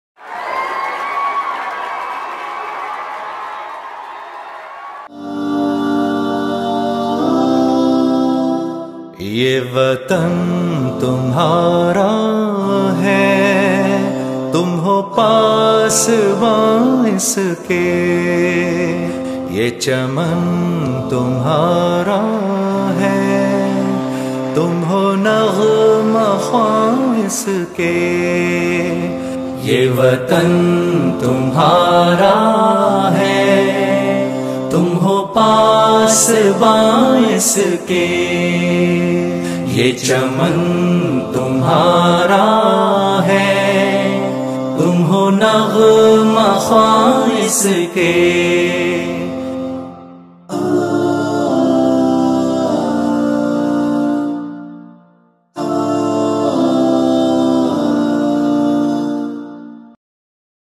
Olympiad'19 Day 2 Highlights
OLYMPIAD'19 Day 2 comes to an end as well. This day was full of excitement with all the sports and academic competitions scheduled for today! Here are the highlights